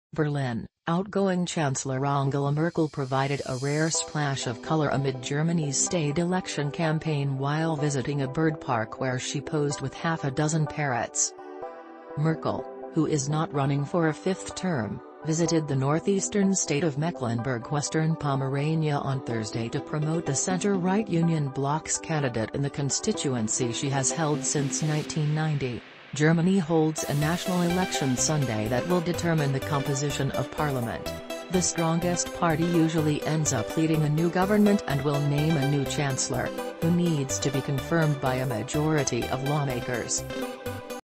Outgoing German Leader Angela Merkel pecked by parrots at bird park
BERLIN — Outgoing Chancellor Angela Merkel provided a rare splash of color amid Germany's staid election campaign while visiting a bird park where she posed with half a dozen parrots. Not content with the bird feed on offer, one of the rainbow lorikeets took a peck at the long-time German leader's hand, prompting a theatrical scream from Merkel. Merkel, who is not running for a fifth term, visited the northeastern state of Mecklenburg Western-Pomerania on Thursday to promote the center-right Union bloc's candidate in the constituency she has held since 1990. Germany holds a national election Sunday that will determine the composition of parliament. The strongest party usually ends up leading a new government and will name a new chancellor, who needs to be confirmed by a majority of lawmakers.